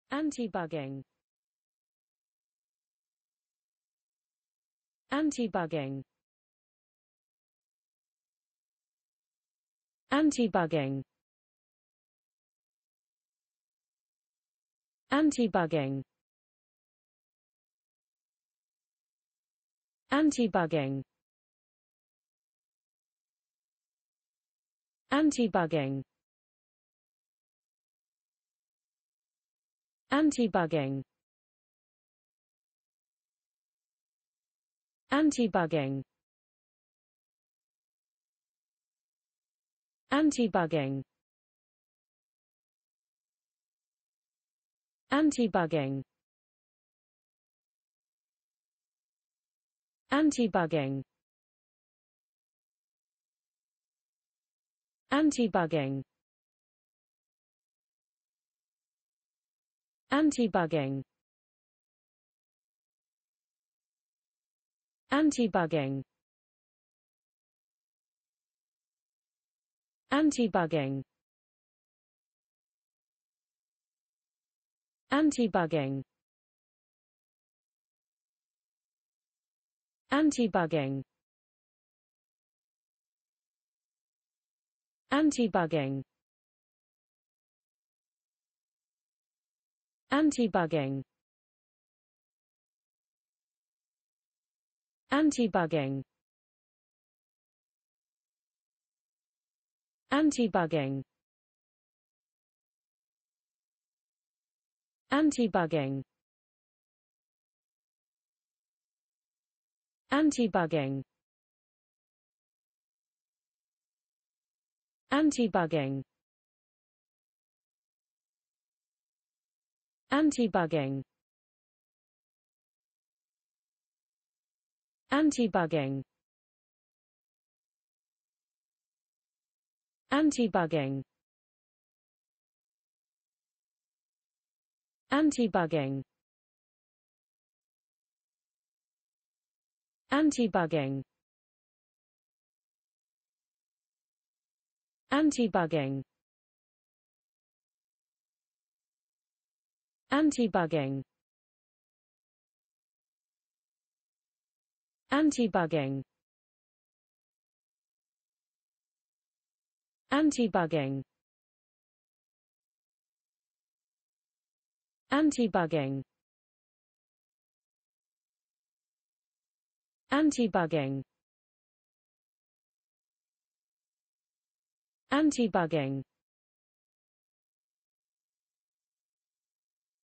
Anti-bugging: How to pronounce Anti-bugging in english (correct!).Start with A. Learn from me.
Follow me!Learn from me how to pronounce the word Anti-bugging in english.This video shows you how to pronounce the word Anti-bugging in english.

Anti-bugging

adj. 防止使用隐藏窃听器的